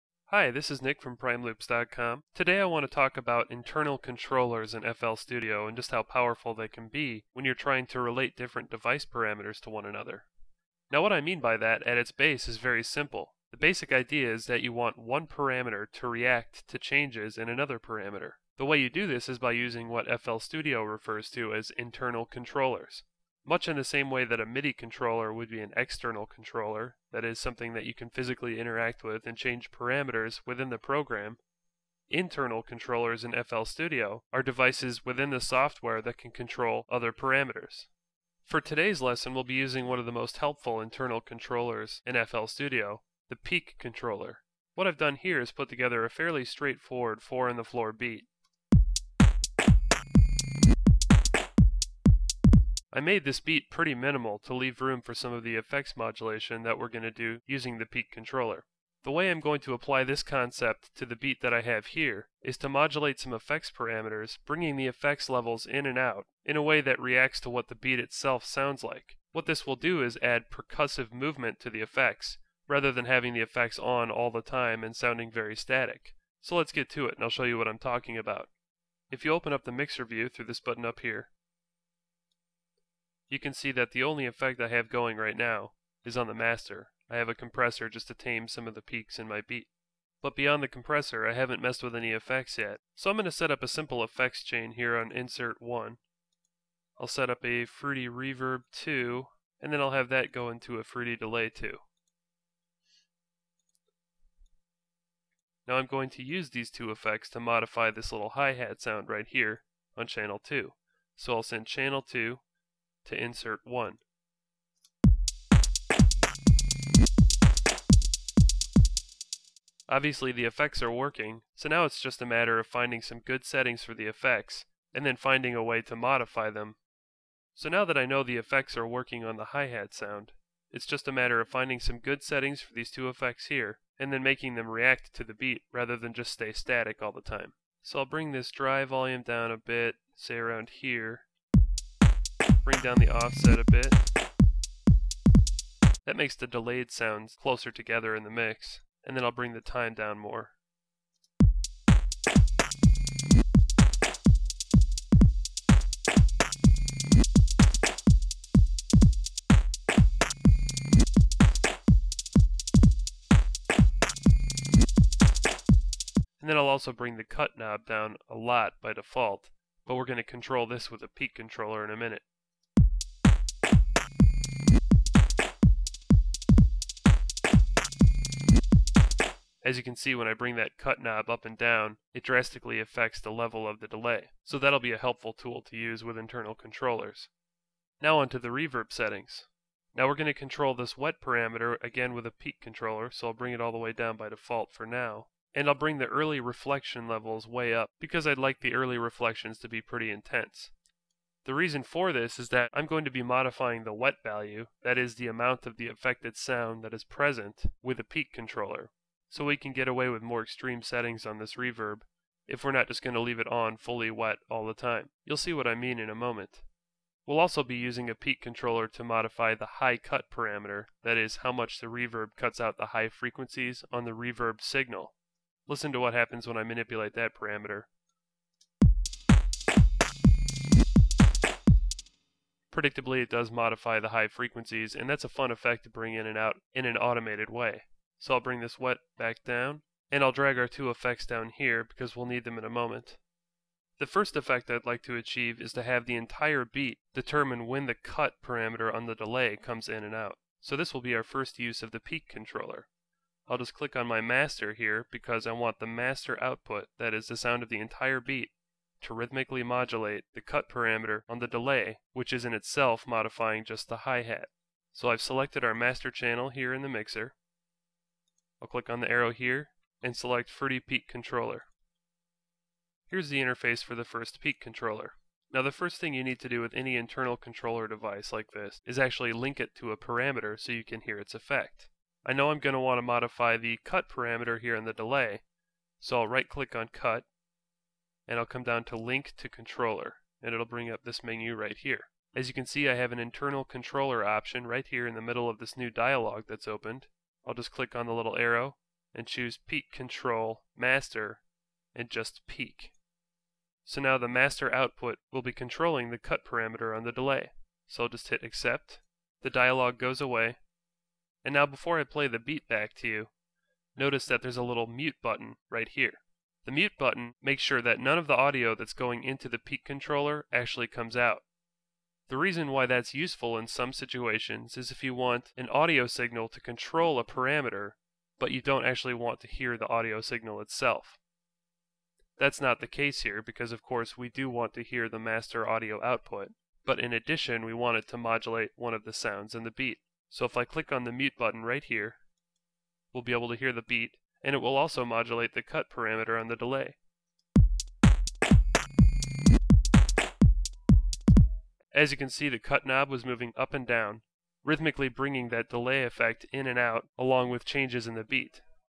FL Studio Tutorial - How To Use Effects [Part 2]
You can download tips and tricks for fl studio for making music. Learn essential music productions techniques including compression, EQ, effects and much more!

FL Studio Tutorial - Using Audio to change Effects Parameters [1]

This is a more advance tutorial for FL Studio which explores assigning and automating effects. FL Studio is a poweful platform, and with the right techniques, endless ammounts of variations are possible!